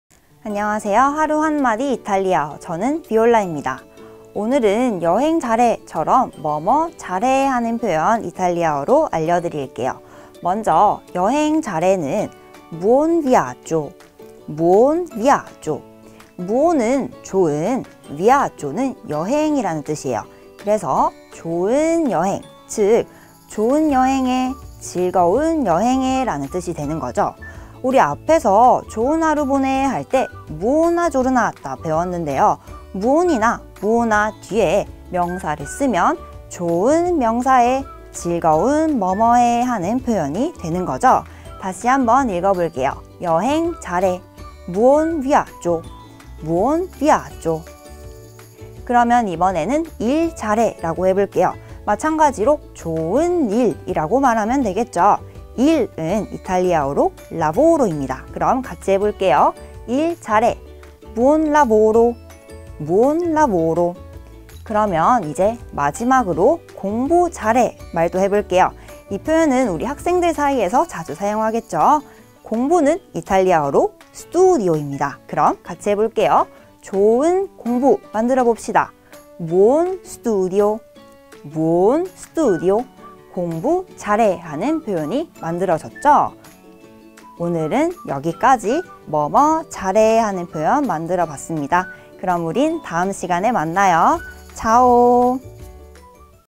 [시원스쿨 이탈리아어] 하루 한마디 이탈리아어-(20)여행 잘해!
[Viola쌤과 함께 말하는 하루 한마디 이탈리아어]

빨리 공항에서 친구, 가족에게 여행 잘해! 라고 말할 수 있는 그 날이 왔으면 좋겠어요🛫
Buon viaggio!